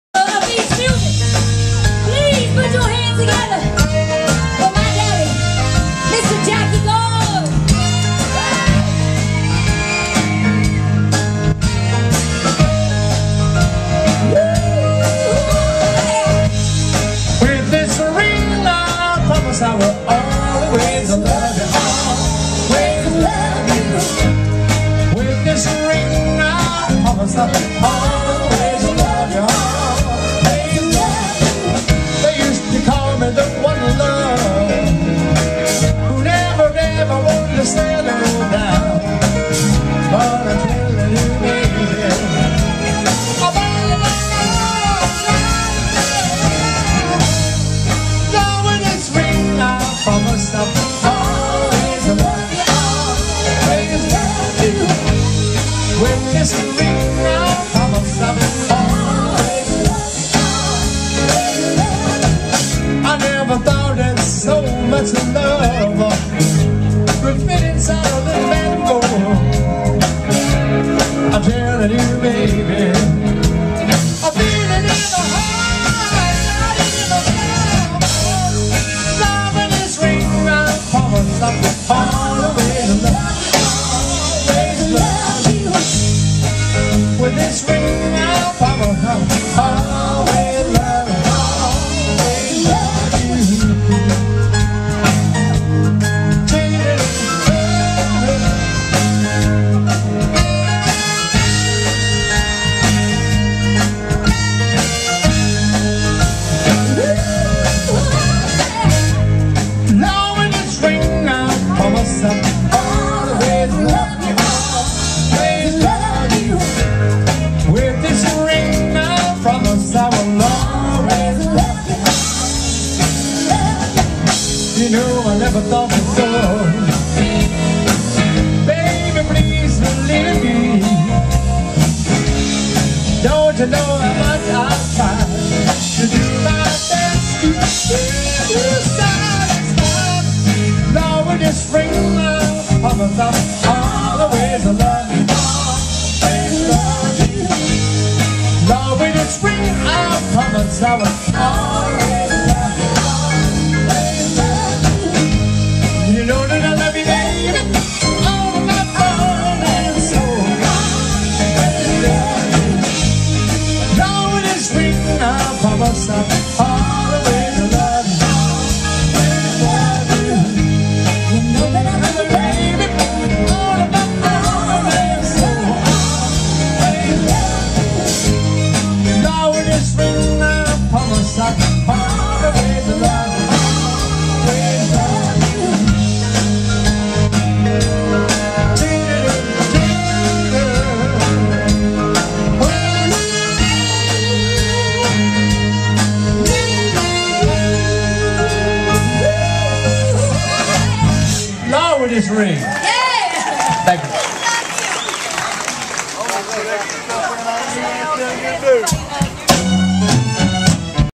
Jackie Gore & band at Belton,SC
Recorded on September 5, 2013 by Jimmy Gilstrap.